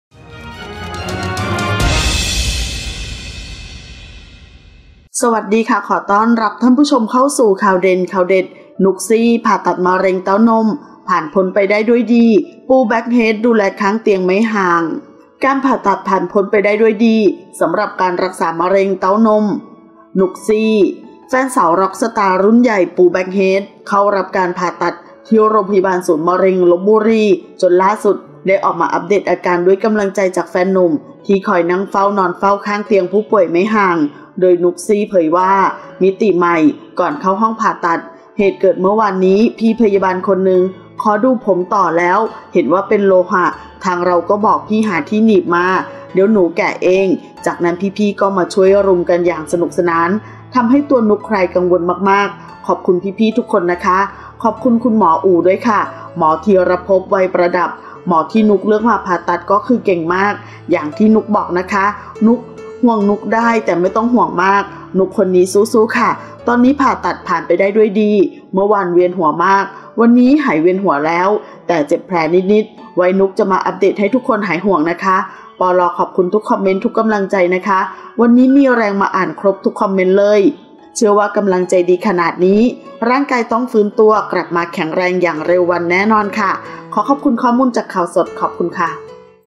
นุ๊กซี่ ผ่าตัดมะเร็งเต้านม ผ่านพ้นไปด้วยดี ปู แบล็คเฮด ดูแลข้างเตียงไม่ห่าง
ขอบคุณที่มา :
ขอขอบคุณข้อมูลจาก: